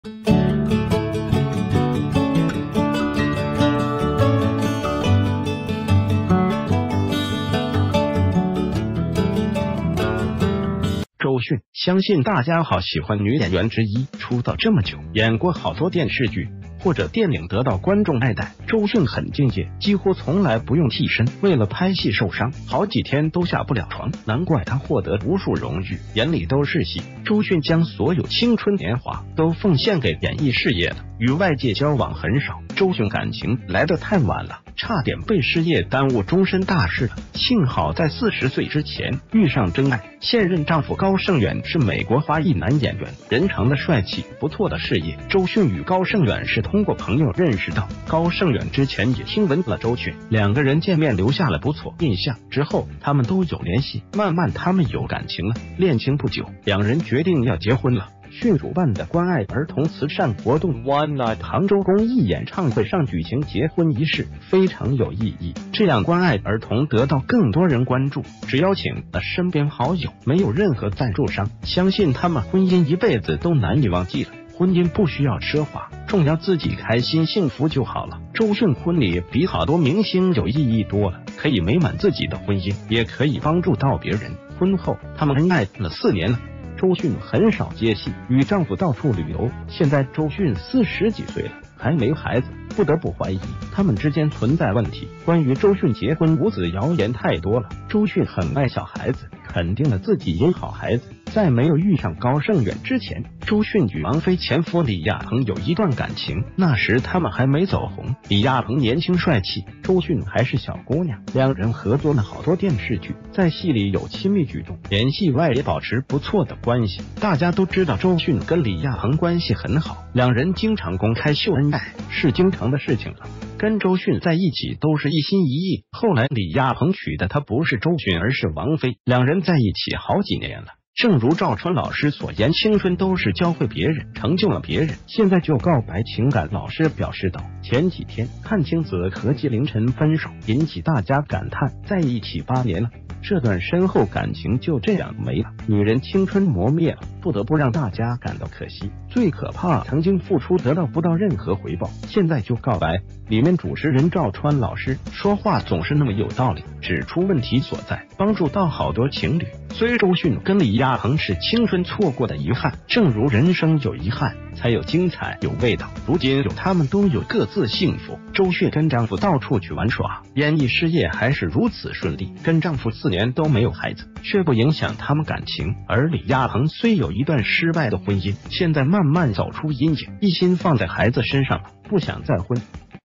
43歲周迅結婚四年了無子，現在真相大白了，原來都是被他耽誤了
周迅，相信大家好喜歡女演員之一，出道這么久，演過好多電視劇或者電影得到觀眾愛戴，周迅很敬業，幾乎從來不用替身，為了拍戲受傷，好幾天都下不了床，難怪她獲得無數榮譽，眼里都是戲。
    周迅將所有青春年華都奉獻給演藝事業了，與外界交往很少，周迅感情來得太晚了，差點被事業耽誤終身大事了，幸好在四十歲之前，遇上真愛，現任丈夫高圣遠是美國華裔男演員，人長得帥氣， 不錯的事業。
    周迅與高圣遠是通過朋友認識到，高圣遠之前也聽聞了周迅，兩個人見面留下了不錯印象，之后，他們都有聯系，慢慢他們有感情了，戀情不久，兩人決定要結婚了，迅主辦的關愛兒童慈善活動one night杭州公益演唱會上舉行結婚儀式，非常有意義，這樣關愛兒童得到更多人關注，只邀請了身邊好友，沒有任何贊助商。
    相信他們婚姻一輩子都難以忘記了，婚姻不需要奢華，重要自己開心幸福就好了，周迅婚禮比好多明星有意義多了，可以美滿自己的婚姻，也可以幫助到別人。
    婚后，他們恩愛了四年了，周迅很少接戲，與丈夫到處旅游，現在周迅四十幾歲了， 還沒孩子，不得不懷疑，他們之間存在問題，關于周迅結婚無子謠言太多了。
    周迅很愛小孩子，肯定了自己也好孩子，在沒有遇上高圣遠之前，周迅與王菲前夫李亞鵬有一段感情，那時他們還沒走紅，李亞鵬年輕帥氣，周迅還是小姑娘，兩人合作了好多電視劇，在戲里有親密舉動，連戲外也保持不錯的關系。
    大家都知道周迅跟李亞鵬關系很好，兩人經常公開秀恩愛，是經常的事情了，跟周迅在一起都是一心一意，后來李亞鵬娶的她不是周迅而是王菲，兩人在一起好幾年了，正如趙川老師所言青春都是教會別人，成就了別人。
    “現在就告白”情感老師表示到，前幾天，闞清子和紀凌塵分手，引起大家感嘆，在一起八年了，這段深厚感情就這樣沒了，女人青春磨滅了，不得不讓大家感到可惜，最可怕曾經付出得到不到任何回報。
    “現在就告白”里面主持人趙川老師，說話總是那么有道理，指出問題所在，幫助到好多情侶，雖周迅跟李亞鵬是青春錯過的遺憾，正如人生有遺憾，才有精彩有味道。
    如今有他們都有各自幸福，周迅跟丈夫到處去玩耍，演藝事業還是如此順利，跟丈夫四年都沒有孩子，卻不影響他們感情，而李亞鵬雖有一段失敗的婚姻，現在慢慢走出陰影，一心放在孩子身上了，不想再婚，怕委屈了孩子。
    好多人青春都有很多遺憾，曾經長久陪伴換來今天離開，也許剛剛開始有遺憾有厭恨對方，不過，不是適合在一起多久還是不適合，不過相信自己遲早能找到自己幸福。